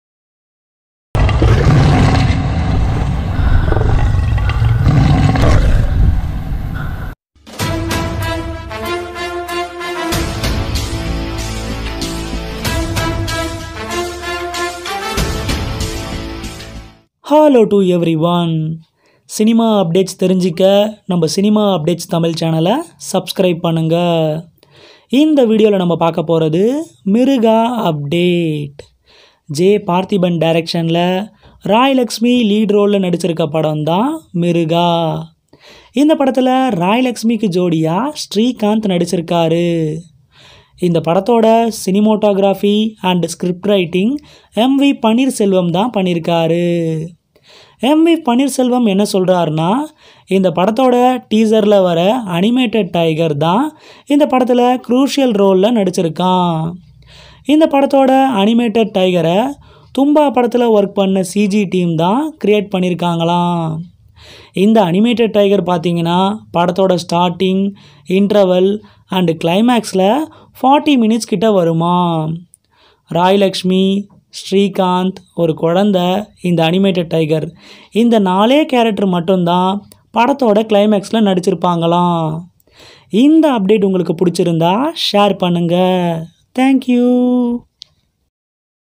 Mirugaa Tiger | Mirugaa Update | Rai Lakshmi | Raai Laxmi | Srikanth
Cinematographer MV Panneerselvam’s debut film as a script writer, Mirugaa, which stars Raai Laxmi and Srikanth, is in its final stages. He had told us earlier that a tiger will be seen in a crucial role in the film, which is directed by J Parthiban.

Now, the team has created an animated tiger which will be seen in the film for almost 40 minutes. “Usually, in our films, the portions that actually have visual effects will be for say, about five to 10 minutes. But in our film, the tiger’s footage lasts approximately 40 minutes,” says Panneerselvam, who has also shot Mirugaa.

The team took several references and made dummies of the tiger, which formed the blueprint for the animated version. “Many filmmakers go to Thailand to shoot with tigers for real, or use rubber or thermocol dummies when shooting scenes with tigers, but we did not resort to any such acts. We have created the tiger in our film entirely digitally. I wanted to do this with an animation studio here rather than go abroad or Mumbai for the visual effects work,” he states.

The studio that he went with was behind the recent film, Thumbaa, which also had a tiger in the central role. Quiz him as to whether the footage created for that film has also been used here, and he says, “You cannot use VFX that was done for a different film because in every shot the lighting will be different, especially when the animated tiger is a character. Work on the visual effects in this film has been going on since March 2019.” He informs that the animal will appear in three sequences in the film.

opening sequence, the interval portion and the 40-minute climax. “The climax will have very little dialogues and will feature just four characters — the hero, the heroine, a child, and the tiger,” he says.